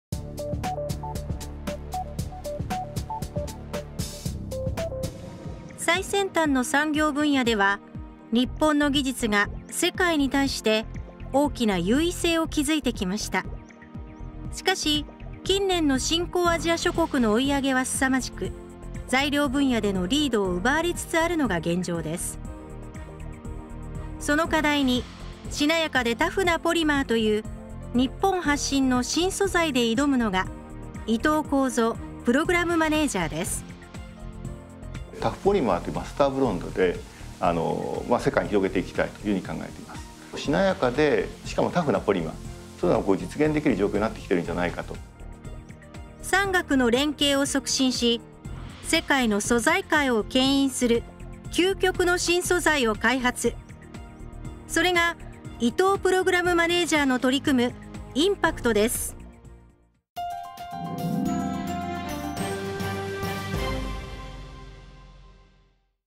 【ImPACTプログラム紹介】 －伊藤 耕三PM－（ダイジェスト版）
ImPACT　「超薄膜化・強靭化 「しなやかなタフポリマー」の実現」（伊藤PM）（ダイジェスト版）